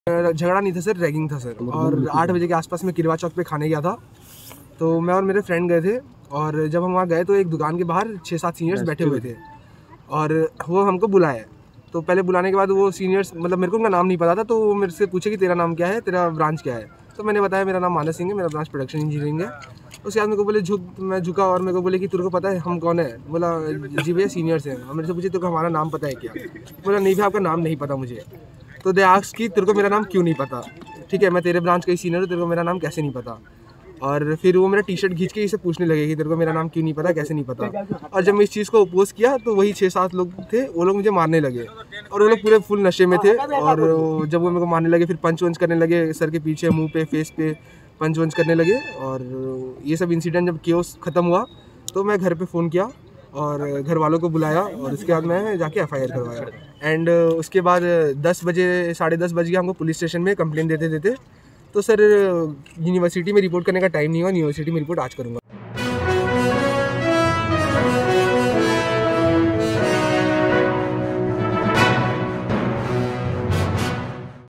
VSSUT Student Alleges Ragging By Seniors, Lodges FIR
Complainant, VSSUT, Sambalpur